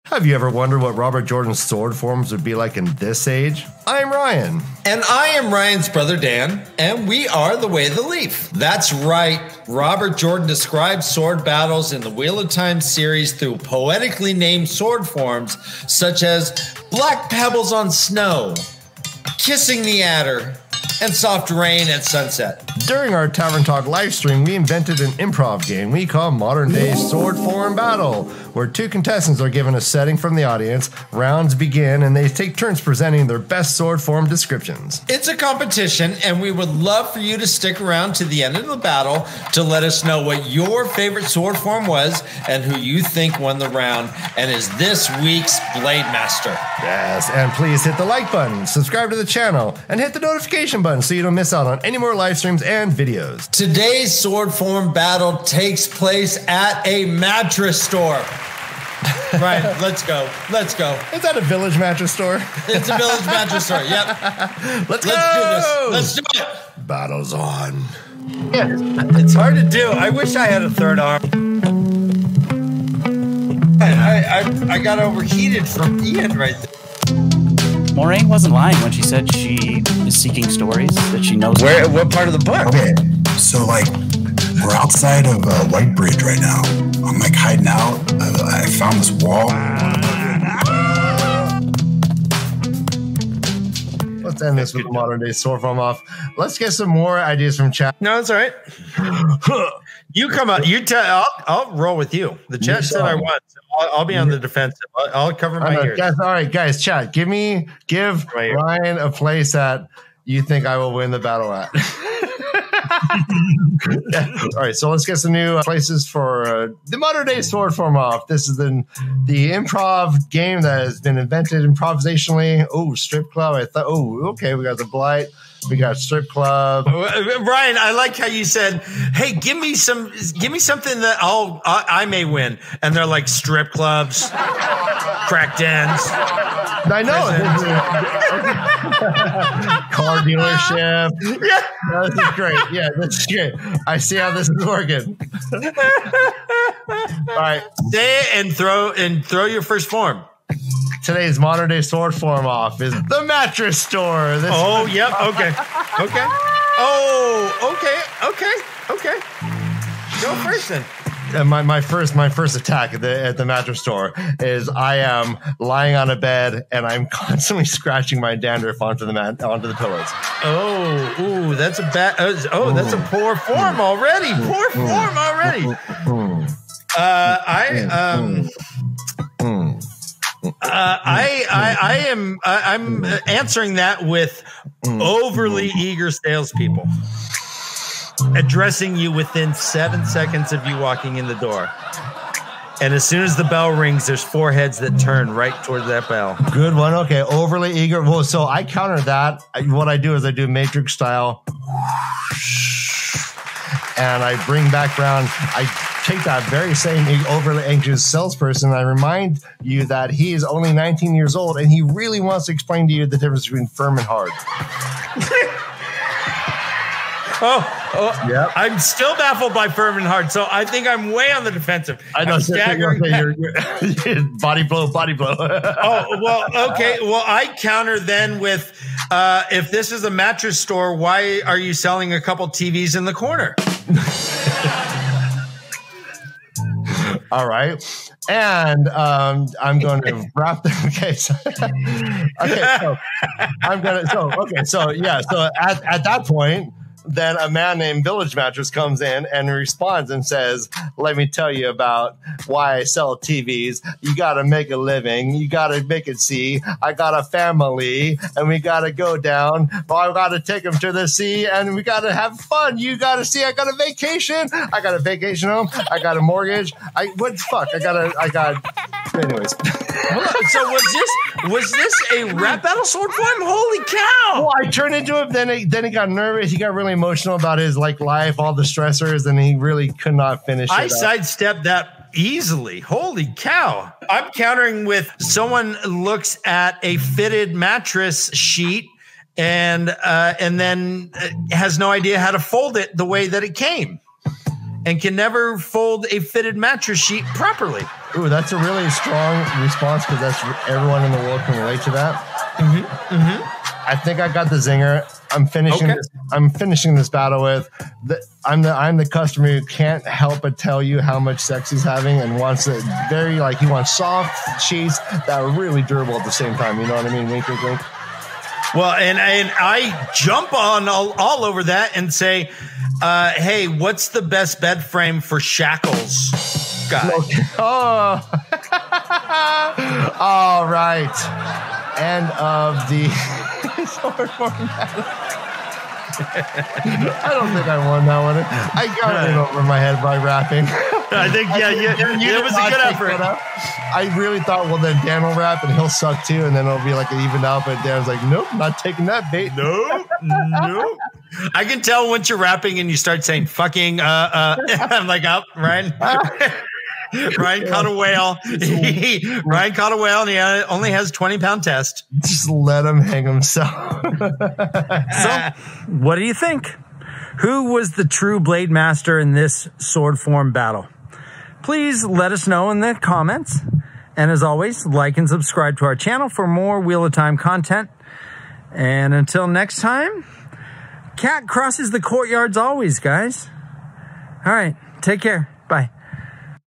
Modern Day Sword Form at the Mattress Store? Wheel Of Time Improv Comedy Sketch.
Robert Jordan wrote battle scenes through poetically named Sword Forms.  This skit is a Modern Take on the Sword Forms!  It IS a competition!!

This video was taken from a live episode of our Tavern Talk shows every Sunday at 7pm EST

Antonello Venditti - Art by Fanucci Editore.